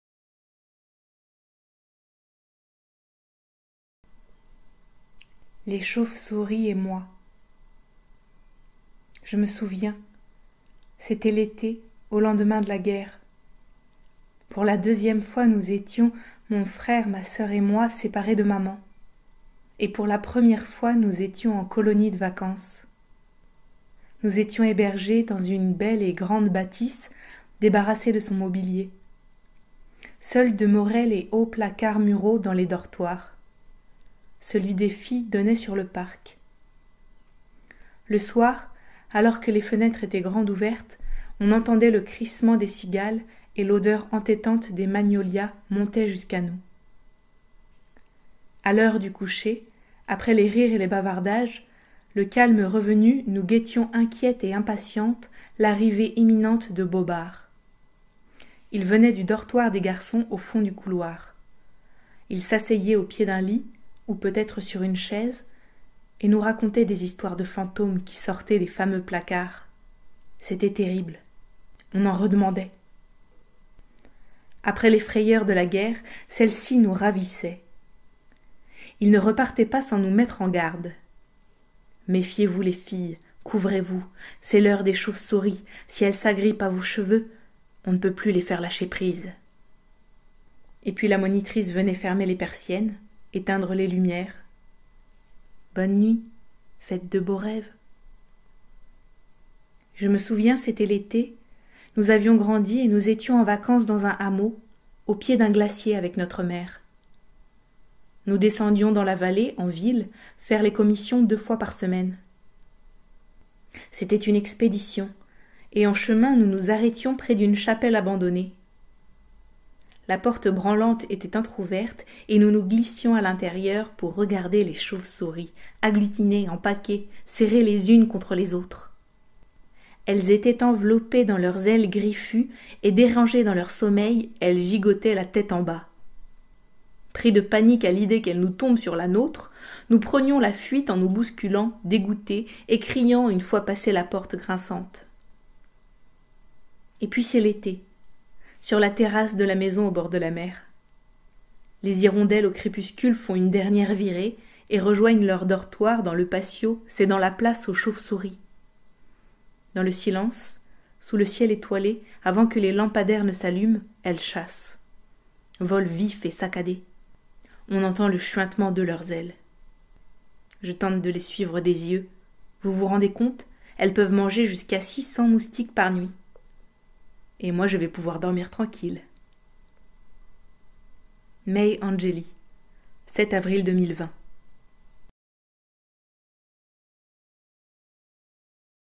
Les chauve-souris de May Angeli lu par Fanny Bayard - Salon du livre d'Arras 2020
Les chauves souris et moi
Texte : May Angeli
Mise en voix : Fanny Bayard